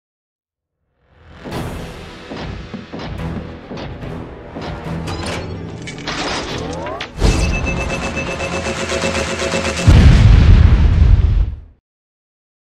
Robot VFX Shot with Arm Cannon Transformation and Pyro
Robot character for my upcoming graduation project

Everything was done by me, myself.

Modelled in Maya, textured in Substance Painter, rendered with Arnold Renderer
Dynamics Simulation (Pyro and Destruction) were done in Houdini FX, with Mantra Renderer